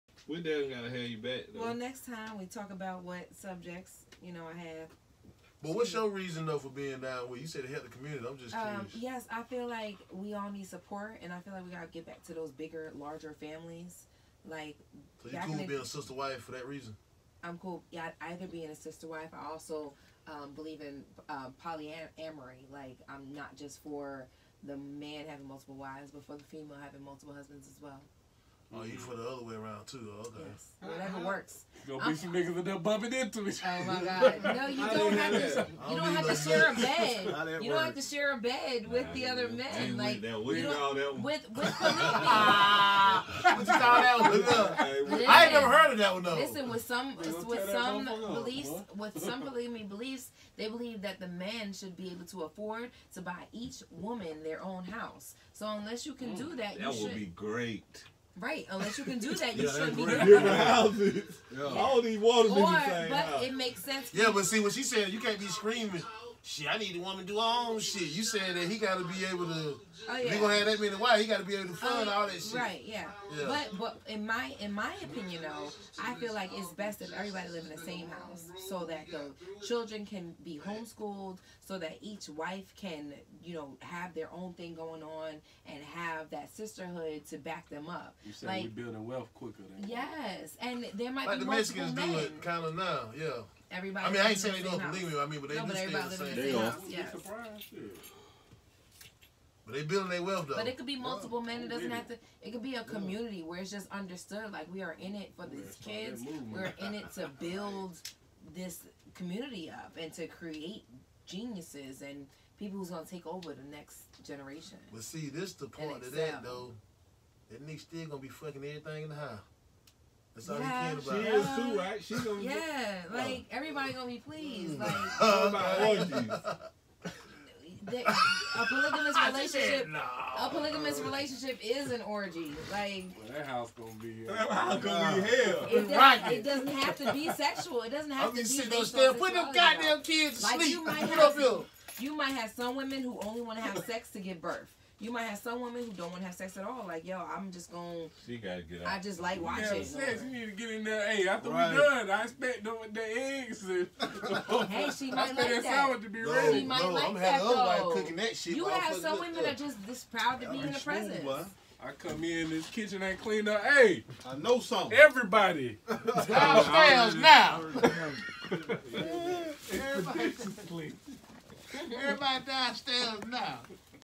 Polygamy ... Behind the scenes with Talk is Talk Podcast
Some extra footage with our guest on Pologamy